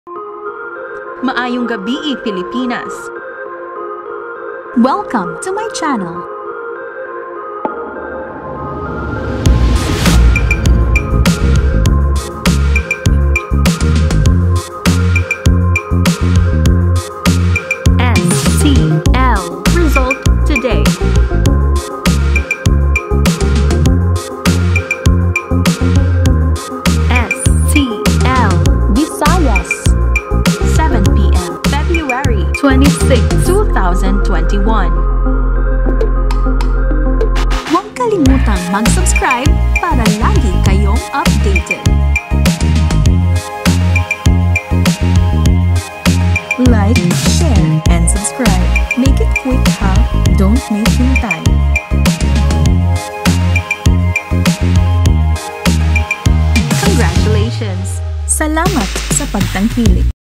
STL RESULT TODAY FEBRUARY 26, 2021 7PM
STL Result Today February 26, 2021 7PM for Mindanao and Visayas area.
STL Mindanao - 7PM Feb 26, 2021
Visayas result draw:
Small Town Lottery Draw are held daily at 1030am, 3pm and 7pm.

0:00 - 0:05 - Greetings
0:06 - 0:15 - Intro Logo
0:16 - 0:55 - STL result today Mindanao and Visayas 7PM February 26, 2021
0:56 - 1:00 - Outro